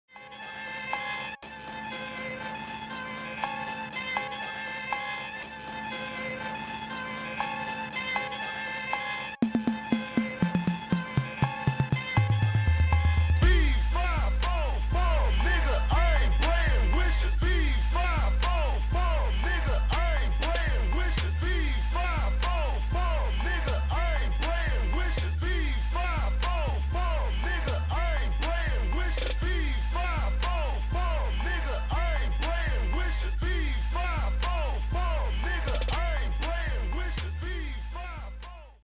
FIFAFOFUM! Trailer (Fingerboard Edit)
this is the Trailer for my first fingerboarding video i hope you enjoy and excuse the Ghettoness of my rail xD